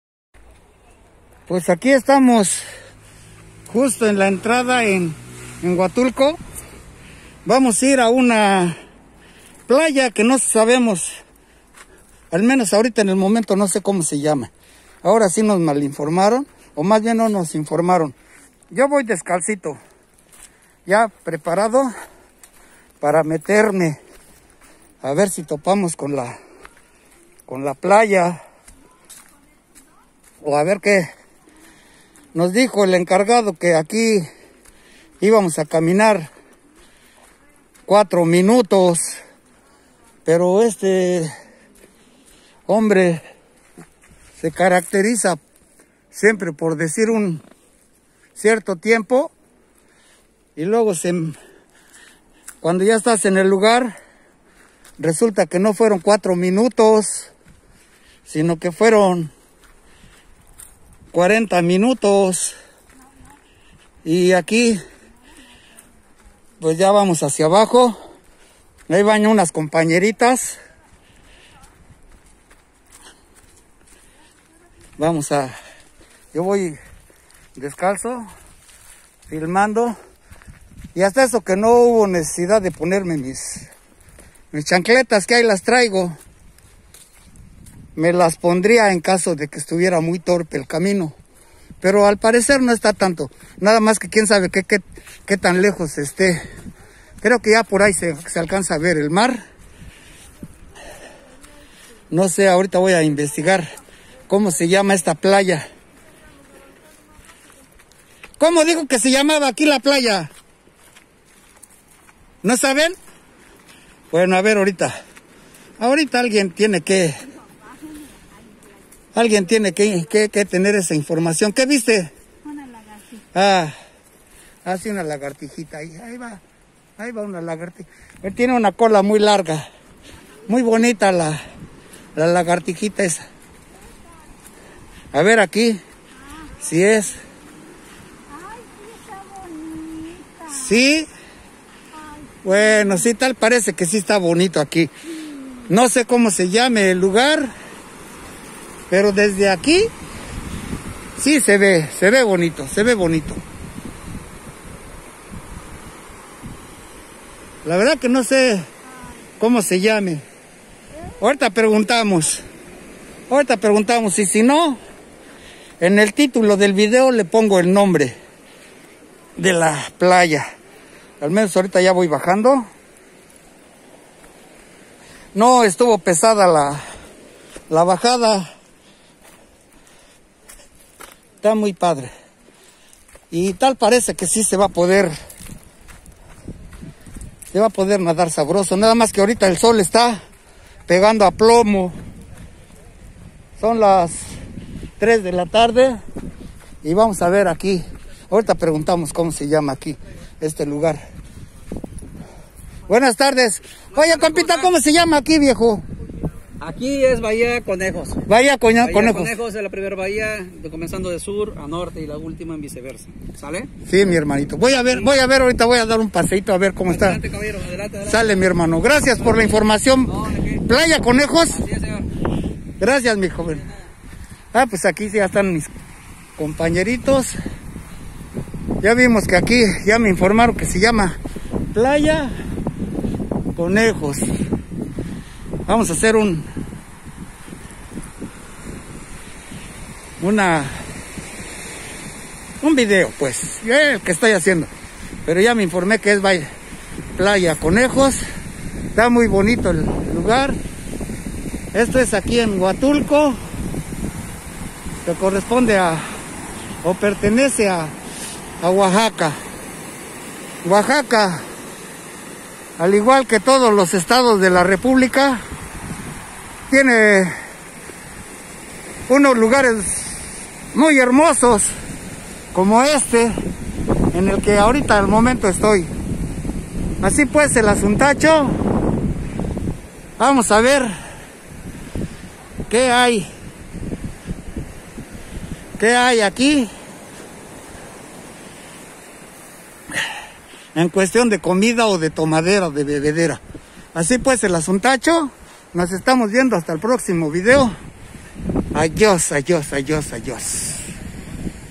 EN BAHÍA CONEJOS, playita muy chida!!!...
Es en Huatulco, Oax, Mex dónde el anfitrión nos trató muy bien y merece ser recomendado!!!...